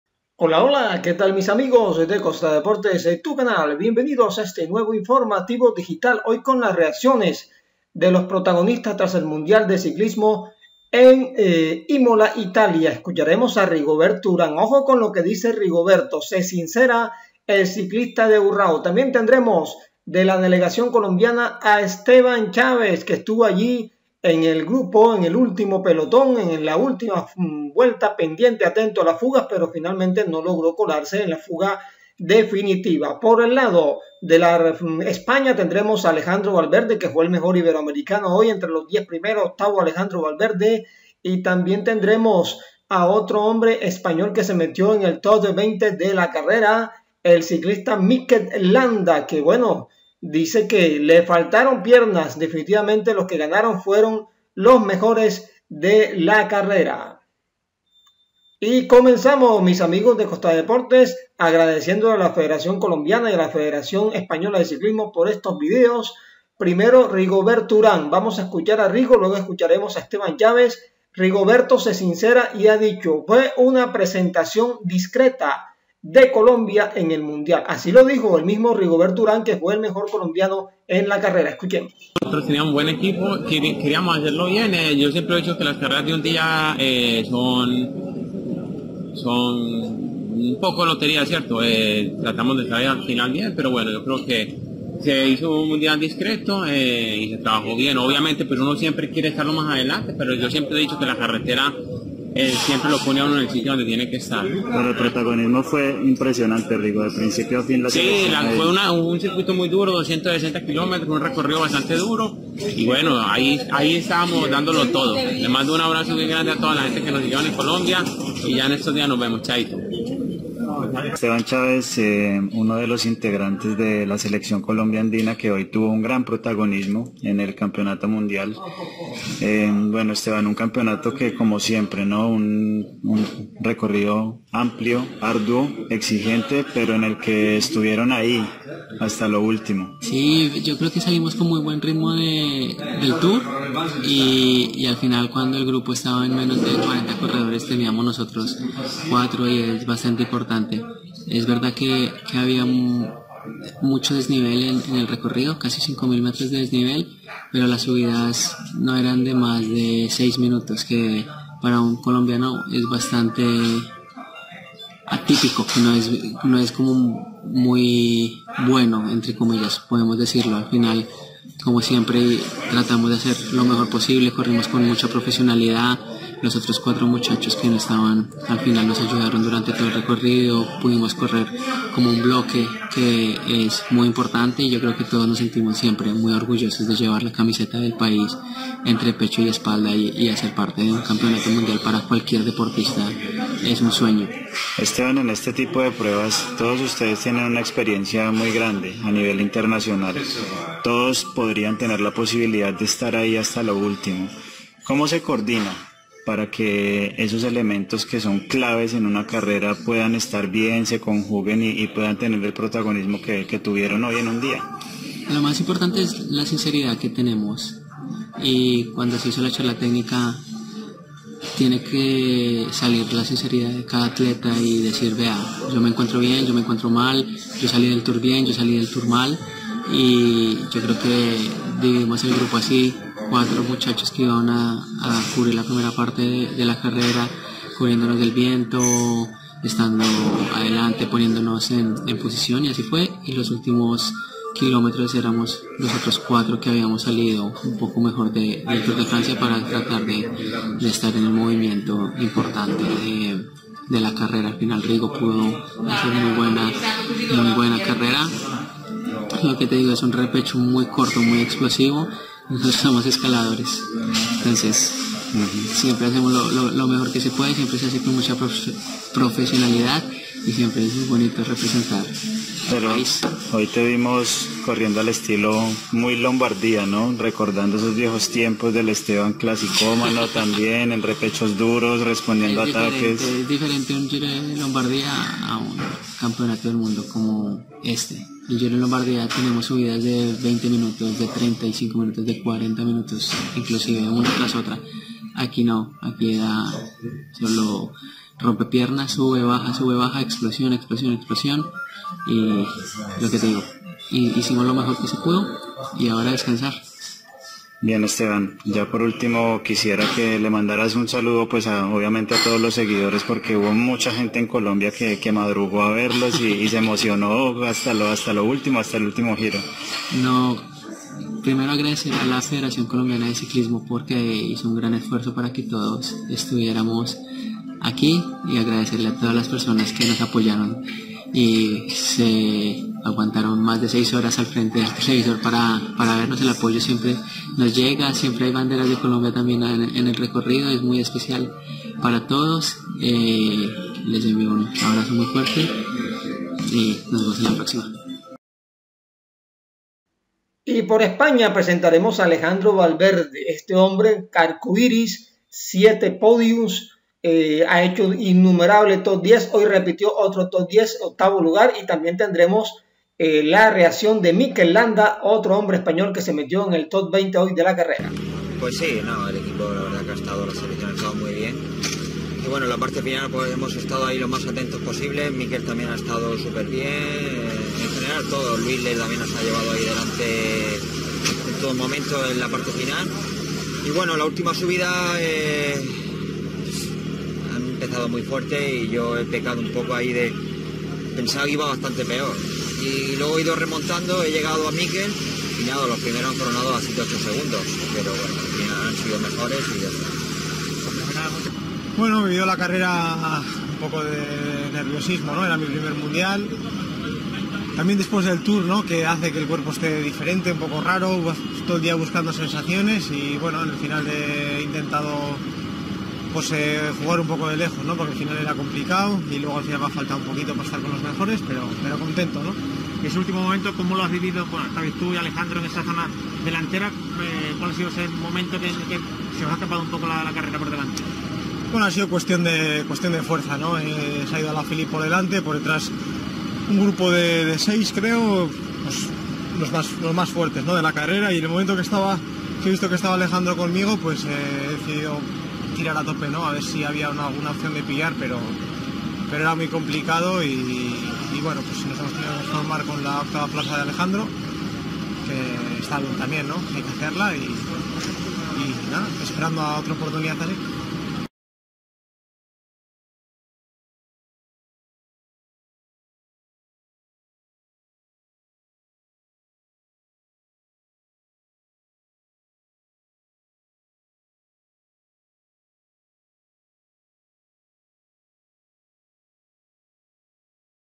RIGOberto URAN y Esteban CHAVES 😦 HABLAN CLARO de lo OCURRIDO con ✅ COLOMBIA en MUNDIAL
RIGOberto URAN y Esteban CHAVES HABLAN CLARO de lo OCURRIDO con COLOMBIA en MUNDIAL Reacciones Alejandro VALVERDE y Mikel LAMDA